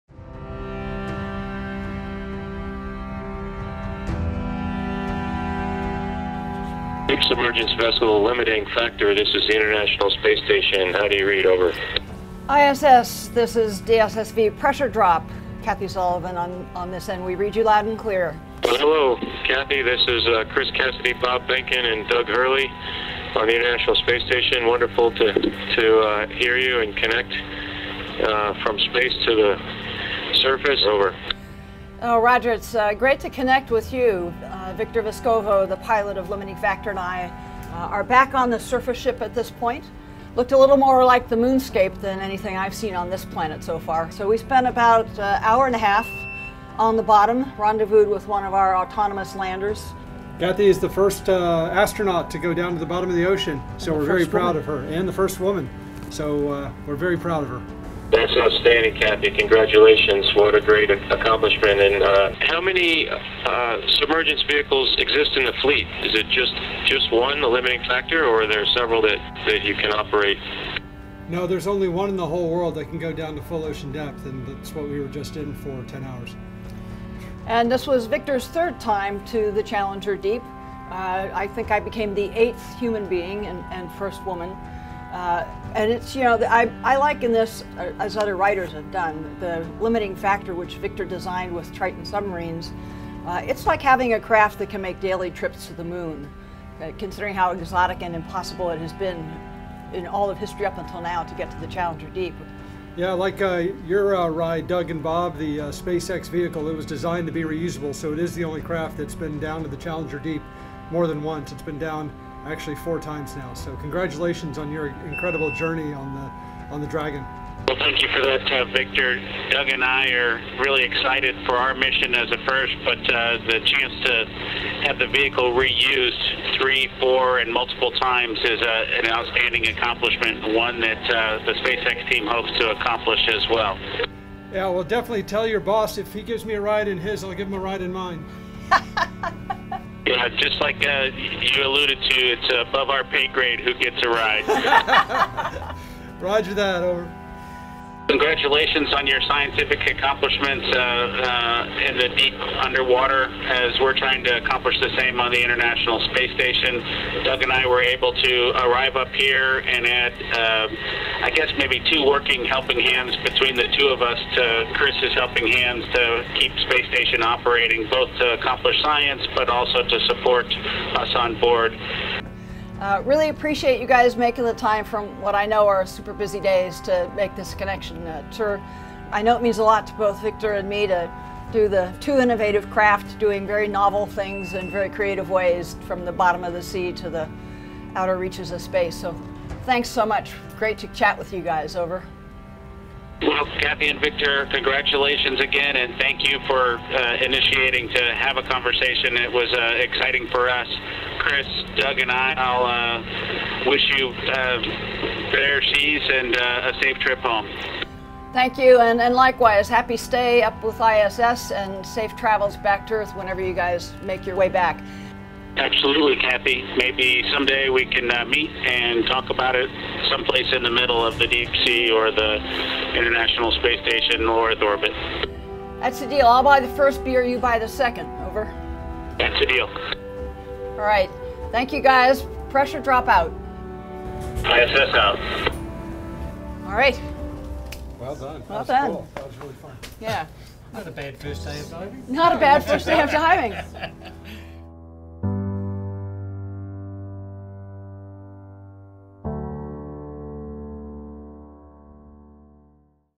Dr Kathy Sullivan speaks to ISS astronauts after historic dive to Challenger Deep
Ex-NASA astronaut Dr Kathy Sullivan is the first woman to both walk in space and descend to the deepest point in the ocean.

After emerging from a successful expedition aboard submersible DSV Limiting Factor (LF) on June 7, 2020, she joined a radio call with astronauts aboard the International Space Station to discuss her historic achievements.

Challenger Deep is the lowest point of the Mariana Trench. Its depth is recorded at between 10,902 and 10,929 metres below sea level.